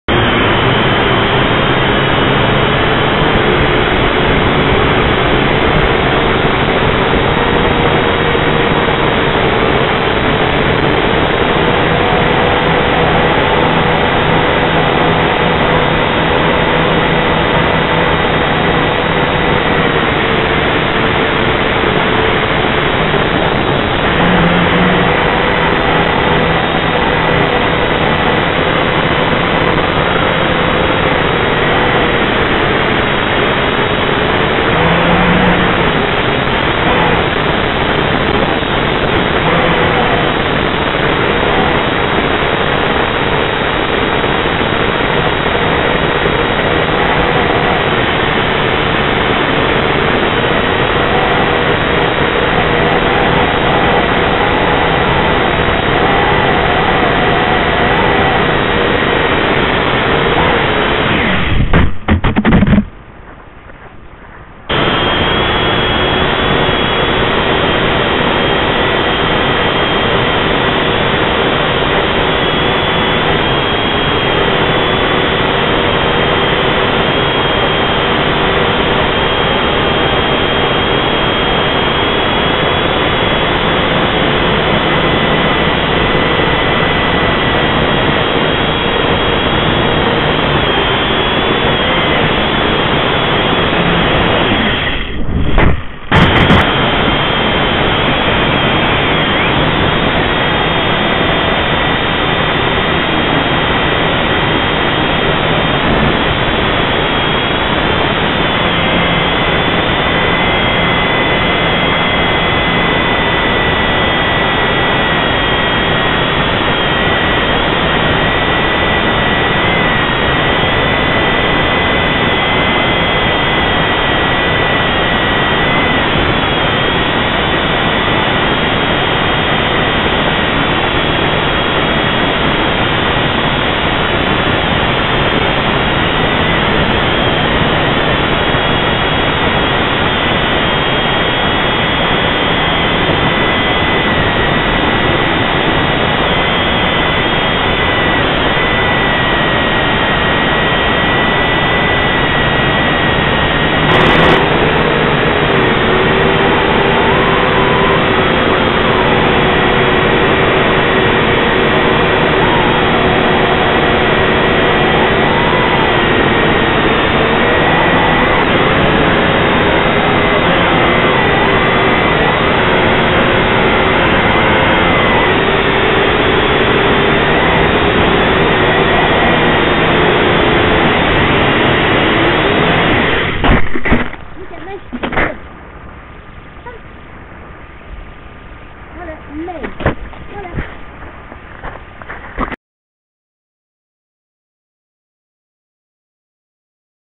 Drönare
Träning på flygning med en drönare som jag köpte i Shanghai på "fake market". Skit kul !!